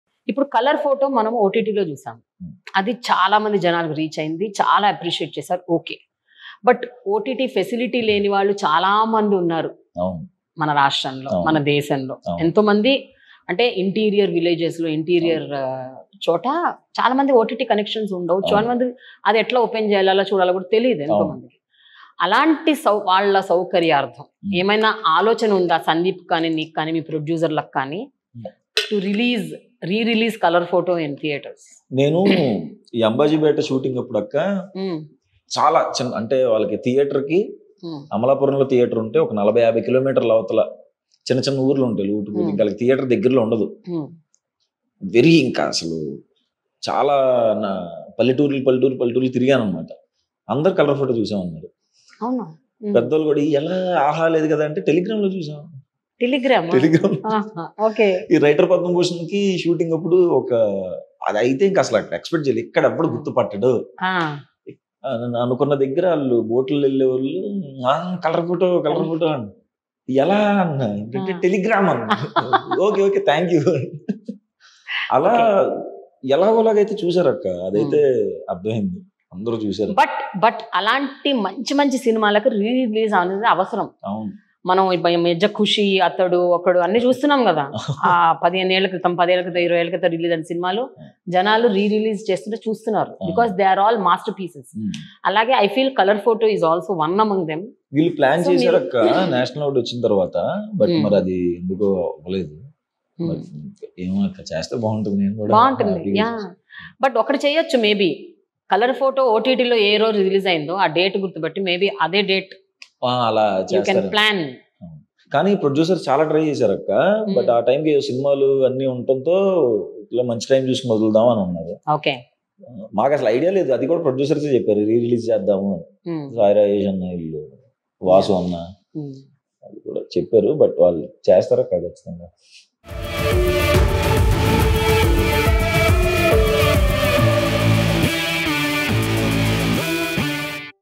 There are plans to re-release Colour Photo | Suhas | Prema the Journalist
Suhas is an Indian actor who works in Telugu films. He initially appeared in short films on the popular YouTube channel Chai Bisket and got his first break with the Telugu movie Majili (2018). He went on to do supporting roles in successful movies like Dear Comrade (2019), Prati Roju Pandage (2019), Agent Sai Srinivasa Athreya (2019), Uma Maheswara Ugraroopasya (2020) before his debut in a lead role with National Award winning movie Colour Photo (2020).

Suhas threw light upon his marriage, inferiority complex, chai bisket journey, Sandeep Raj, memorable moments with actors, upcoming movie Writer Padmabhushan and much more in this colourful, lively and grounded conversation with Prema The Journalist.

Edited & GFX by: Gopi Kanth Nuthi
Editor: Ramakrishna reddy Arram
Camera: Samuelsai, Anil Nunjala